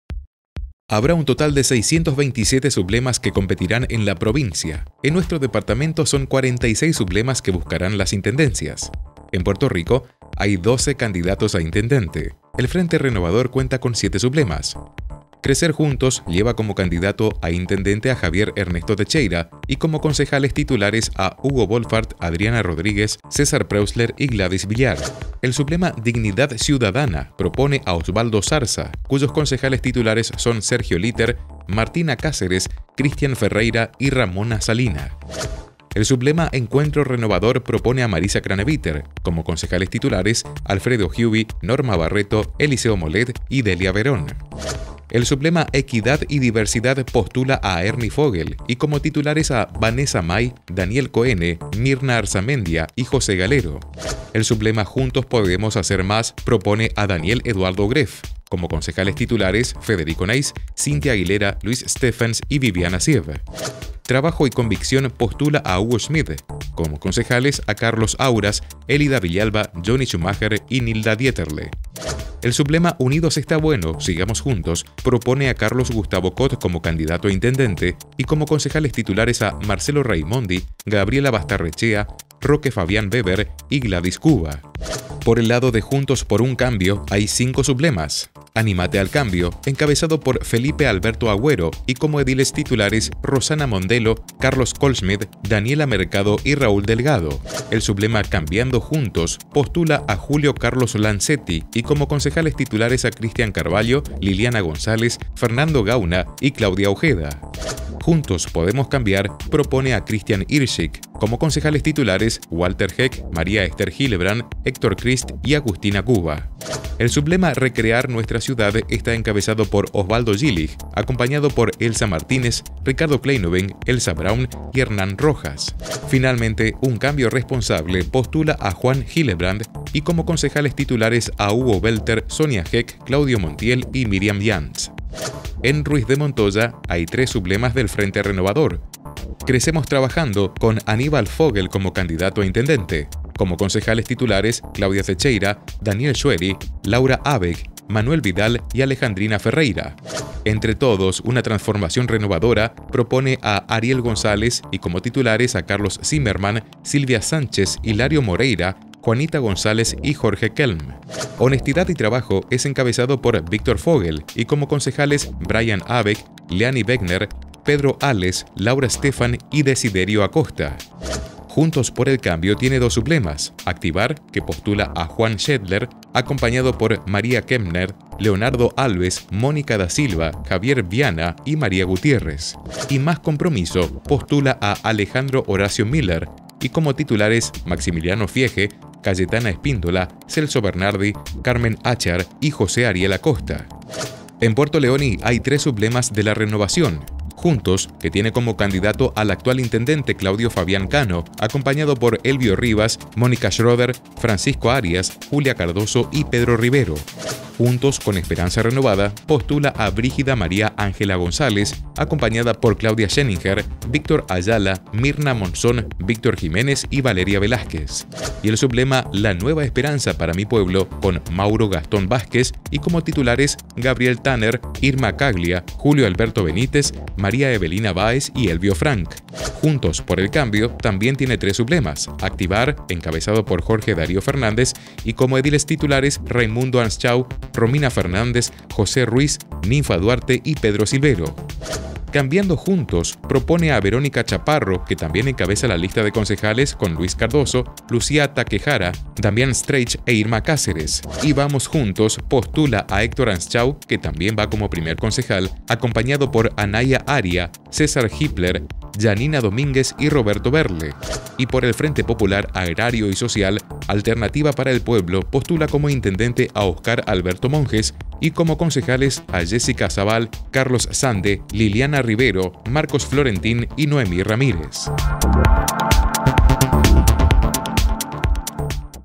Elecciones 2019: Candidatos de Puerto Rico, Ruiz de Montoya y Puerto Leoni
Canal 3 - Puerto Rico, Misiones, Argentina
El Tribunal Electoral difundió la nómina de candidatos municipales para las elecciones. Habrá 12 candidatos a intendente en Puerto Rico, 5 en Ruiz de Montoya y 7 en Puerto Leoni.